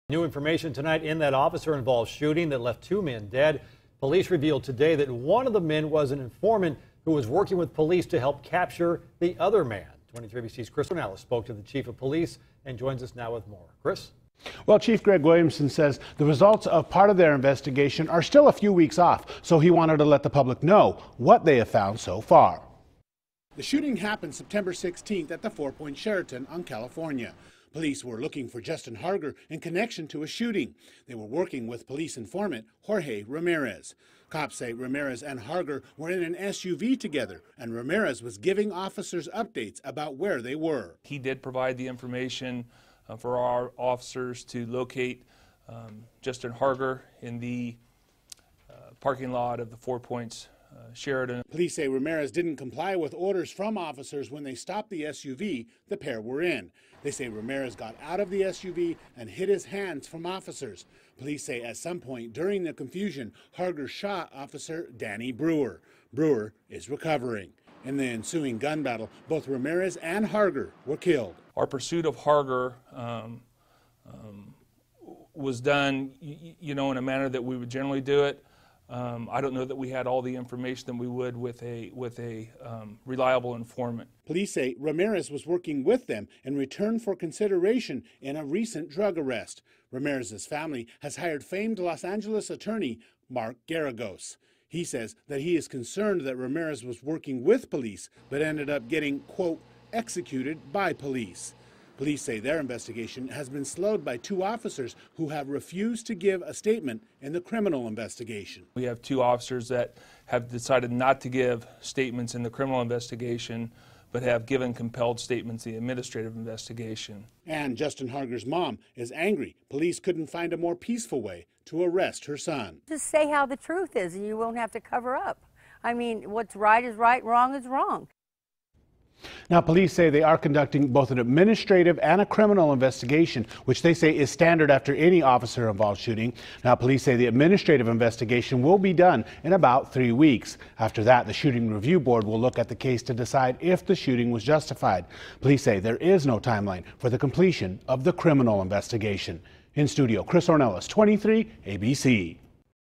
Informant killed by police in shootout
BPD said one of the men killed in a shootout was an informant.